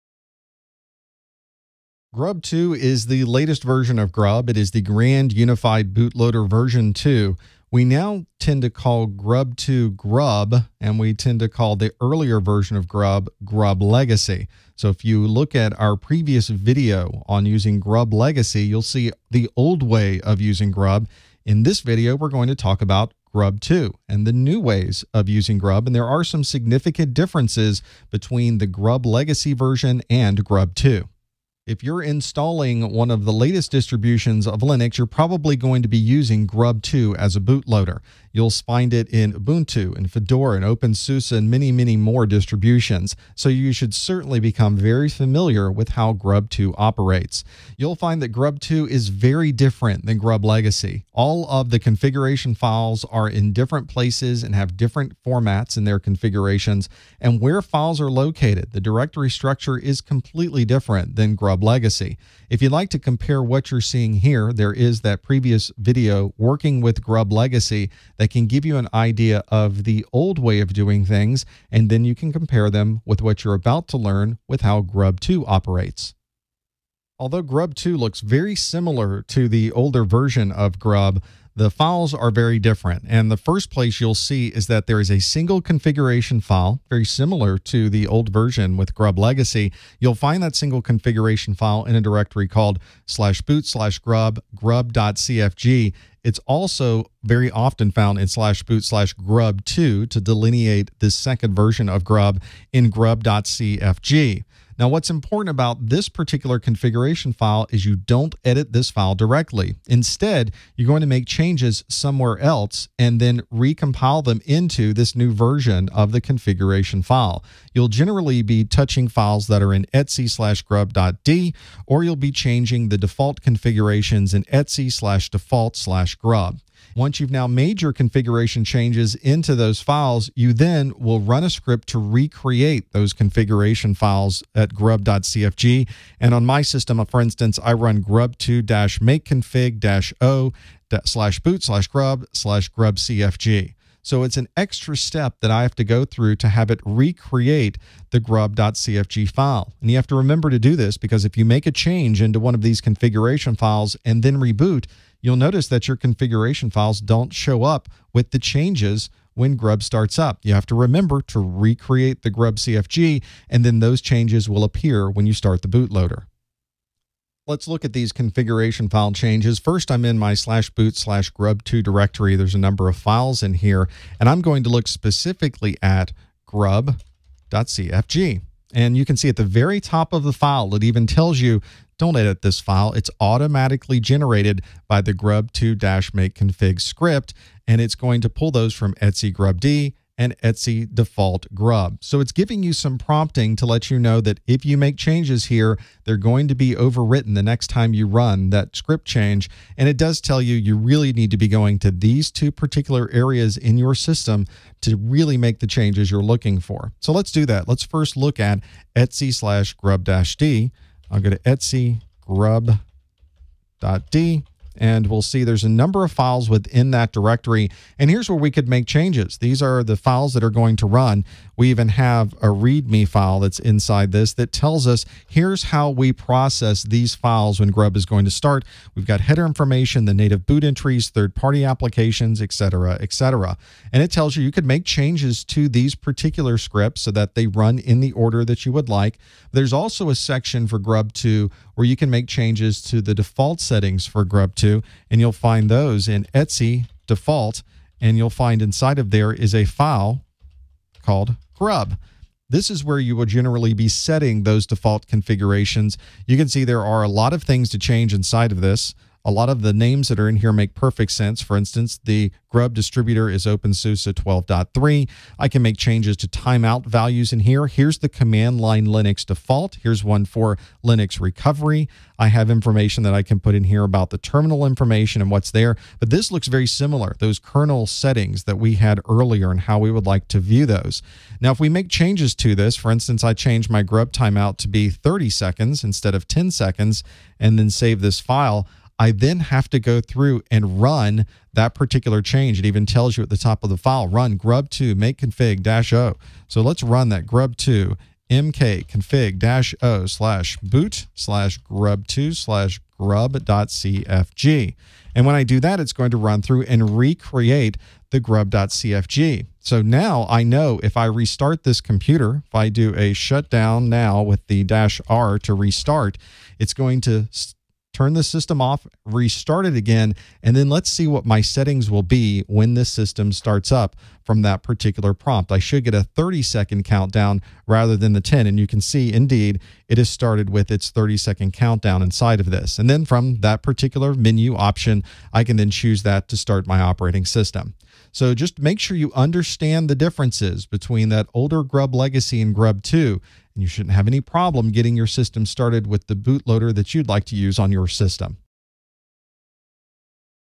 Working with GRUB 2 - CompTIA Linux+ LX0-101, LPIC-1: 102.2
GRUB 2 is the update to GRUB Legacy, and it includes a number of enhancements. In this video, you'll learn about the advantages and differences when running a GRUB 2 as your bootloader.